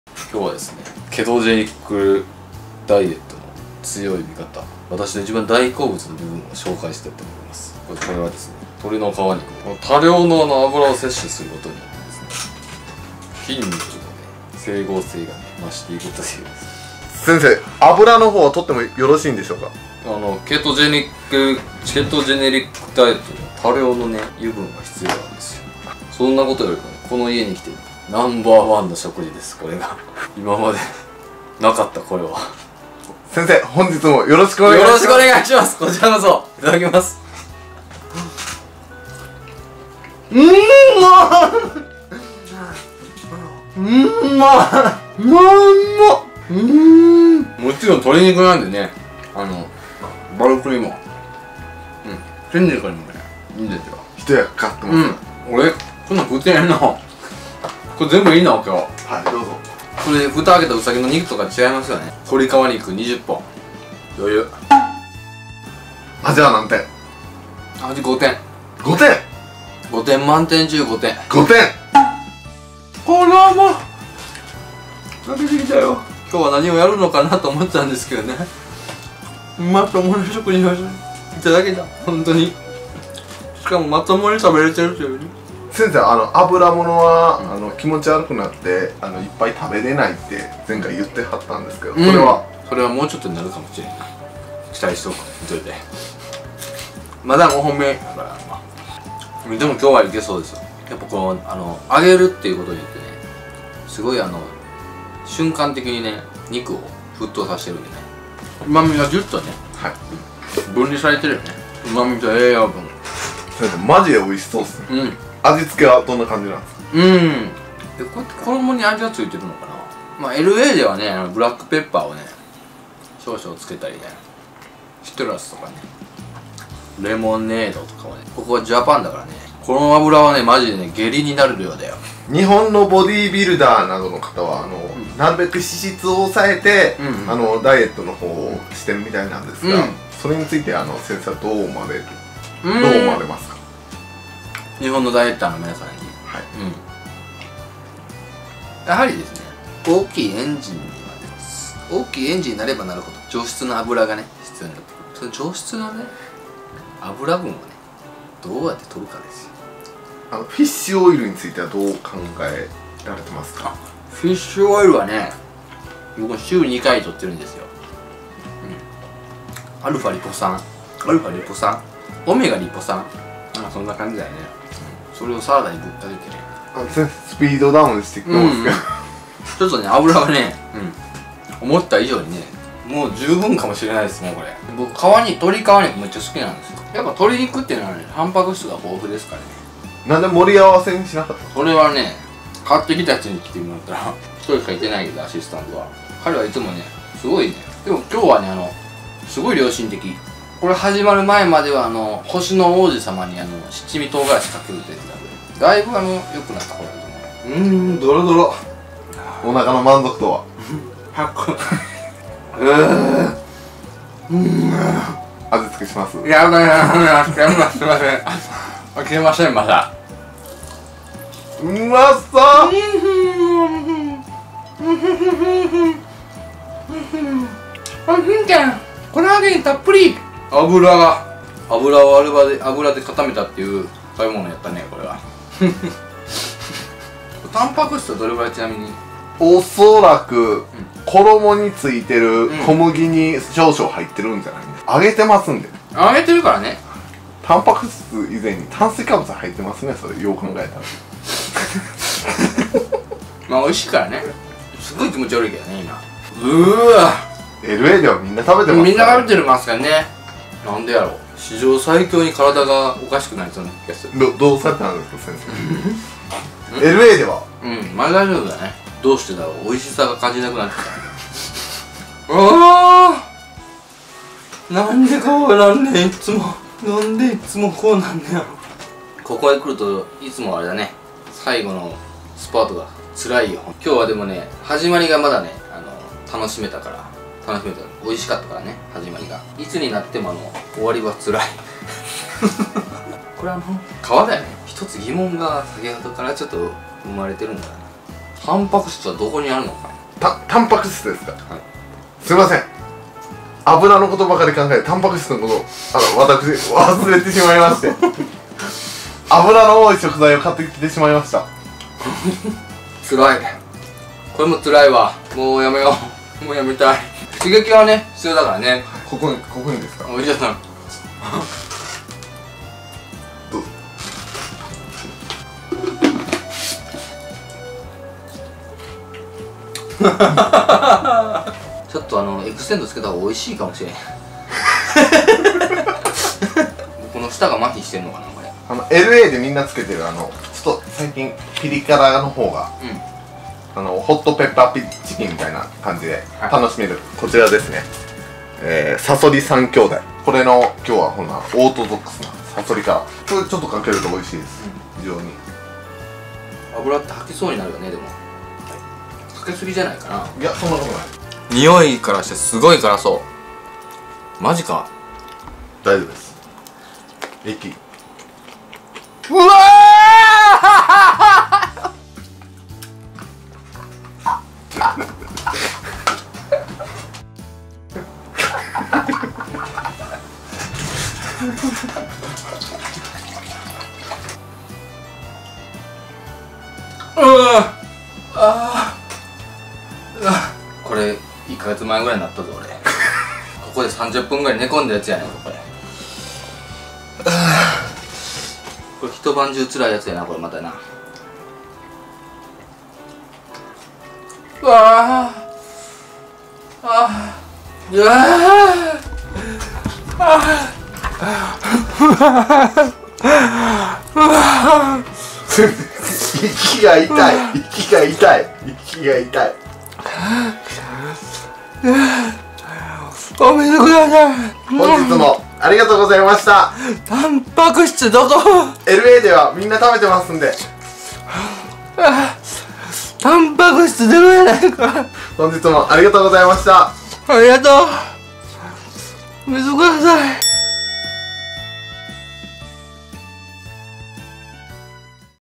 ケトジェニックダイエットに必要な油を効率よく摂取する匠先生｜鳥皮揚げ20本タンパク質ほぼ無し
北島先生もびっくり匠のケトジェニックダイエットの食事風景